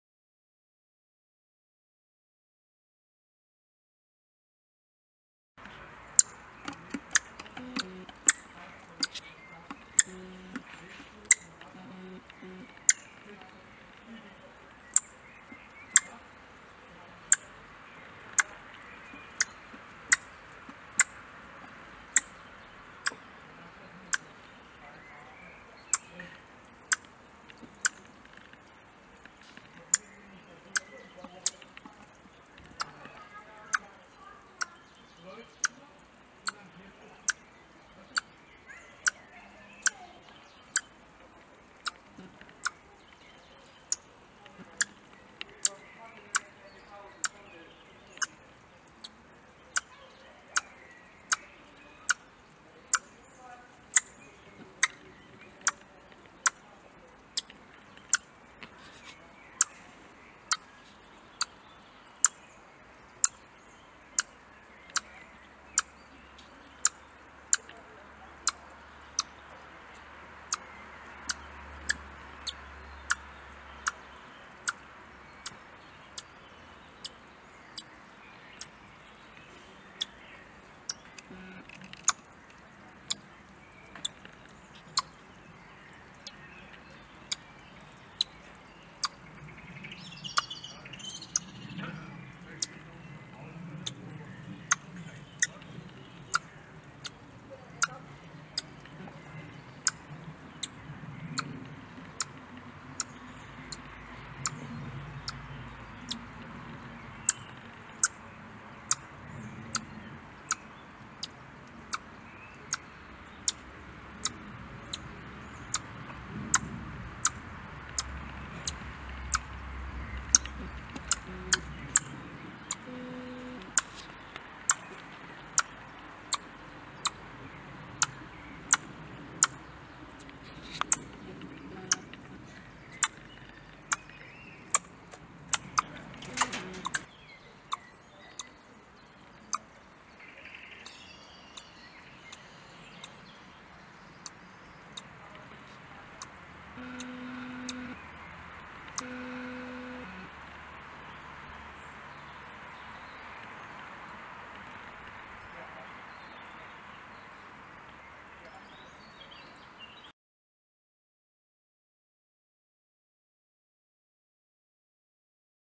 Bibi regt sich auf- Eichhörnchen Handaufzucht Tag 49 - Squirrel Baby
Bibi hat meine Katze im Garten vor ihrer Auswilderungsvoliere gesehen. Das gefällt ihr gar nicht ;-)Infos zur Aufzucht von Eichhörnchen findest du auf meiner privaten